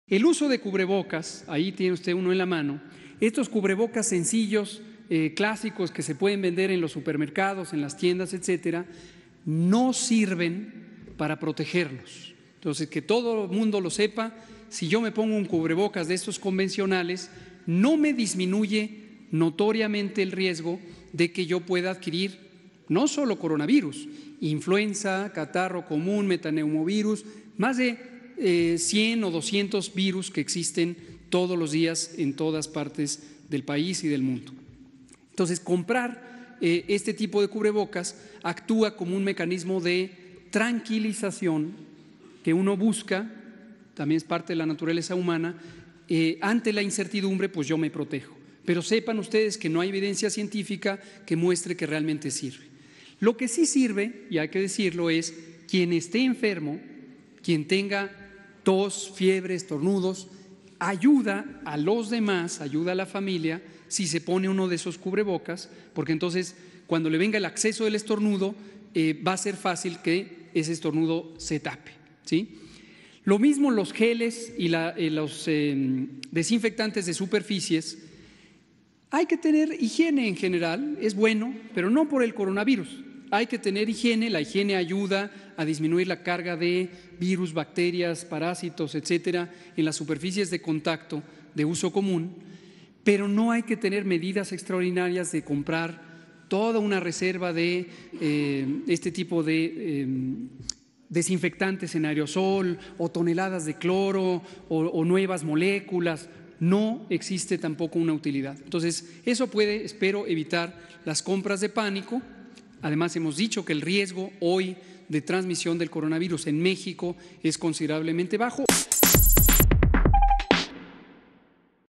Sin evidencia científica que demuestre que funcione el uso de cubrebocas: López-Gatell
Hugo López-Gatell subrayó «no hay evidencia científica para demostrar que los cubrebocas sirven para evitar el contagio del coronavirus, en general, hay que tener higiene ya que ayuda a disminuir la carga de bacterias en la superficie común».